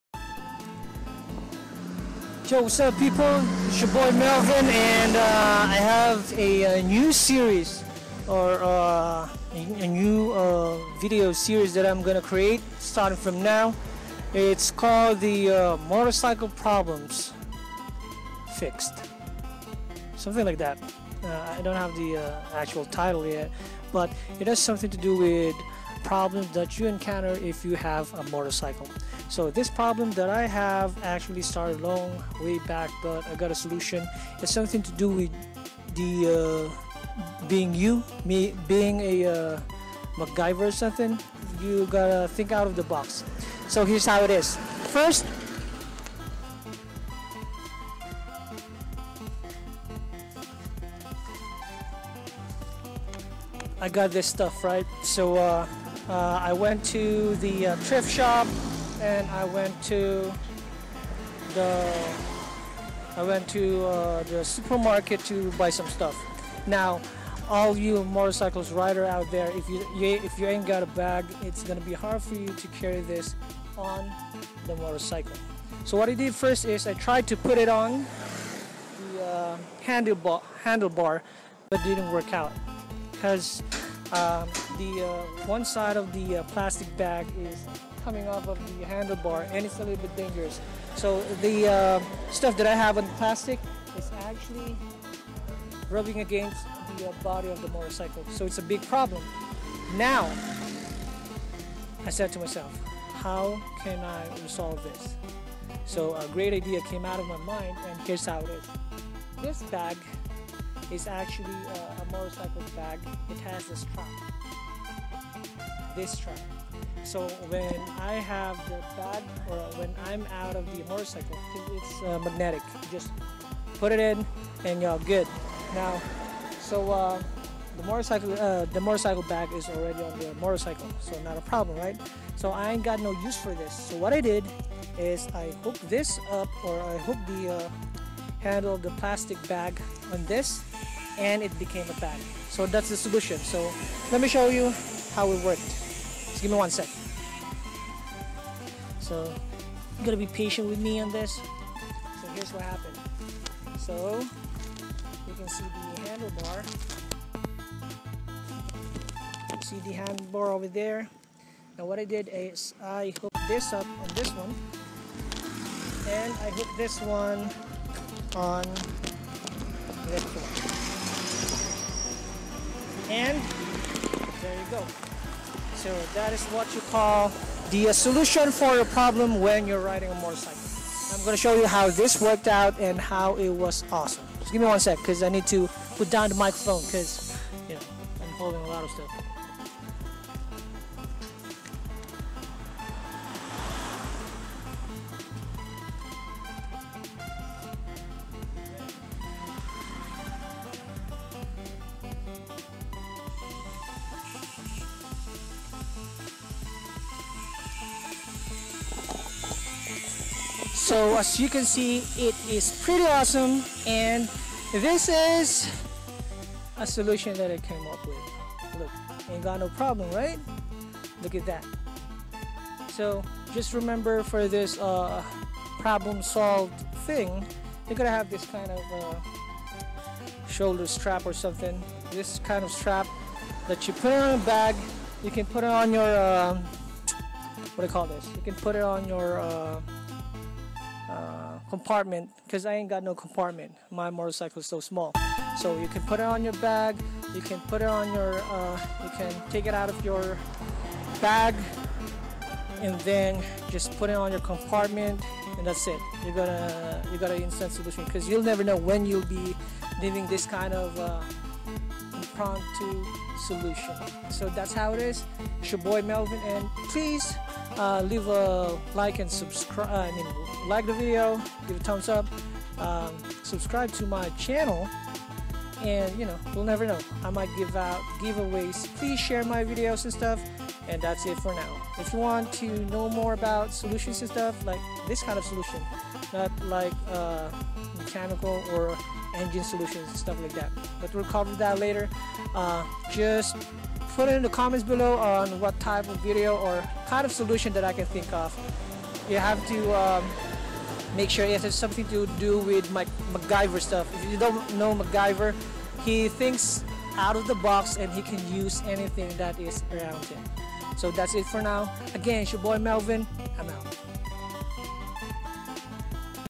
Motorcycle Problems Solved - Carrying Stuff
I found a solution when you are in having a problem with carrying stuff when riding a motorcycle.